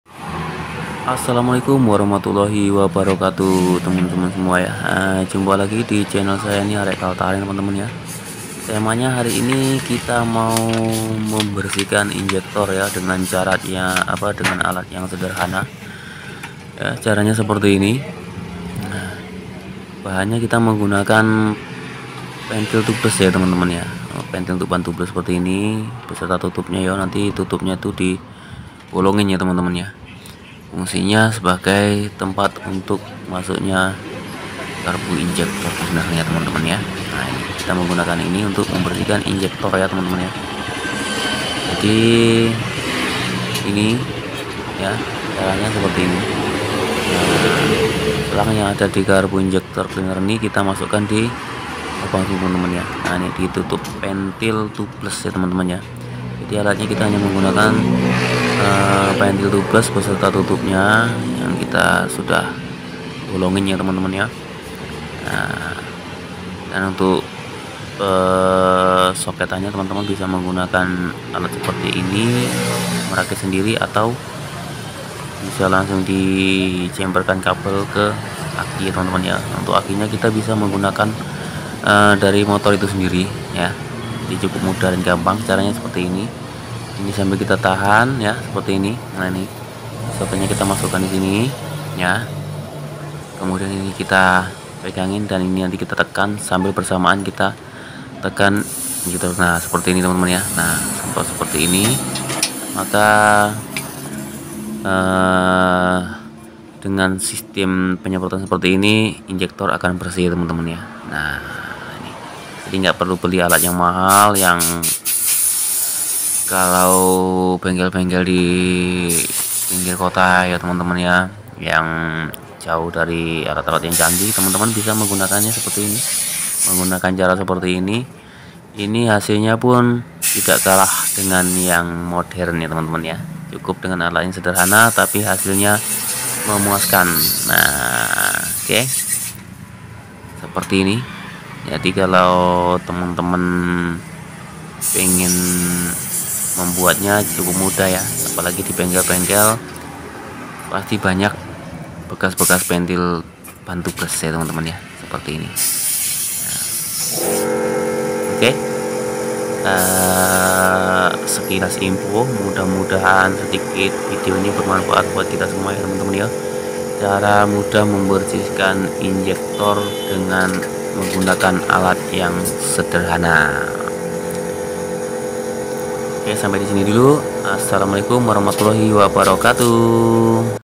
cara membersihkan injeksi secara manual
dalam vidio ini menjelaskan cara membersihkan injeksi secara manual,selamat menyaksikan.